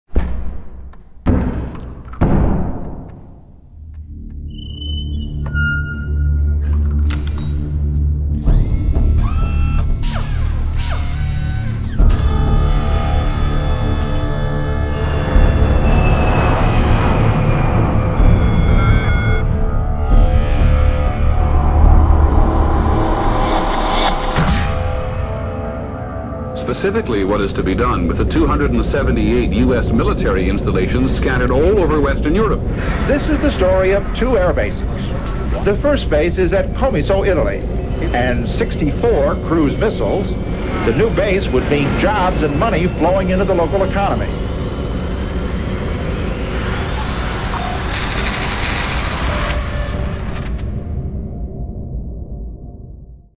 Comiso Air Base Today 2020 (Anteprima)
Up to 28 years ago, the largest military base in southern Europe was located here in Comiso, where during the Cold War it kept under Soviet military control. The structure, in an area now abandoned to all types of parking, was chosen by the United States to house 112 nuclear warhead cruise missiles. It is now a ghost town with houses, schools, kindergartens, and much more.

Qui a Comiso fino a 28 anni fa aveva sede la più grande base nato militare di tutta l'Europa meridionale dove in piena Guerra Fredda teneva sotto scacco l'Unione Sovietica. La struttura, in un'area oggi abbandonata ad ogni tipo di saccheggio, era stata scelta dagli Stati Uniti per custodire 112 missili Cruise a testata nucleare. Adesso è una città fantasma con case, scuole, asili, e molto altro ancora.

All rights are reserved - 2020 COMISO AIR BASE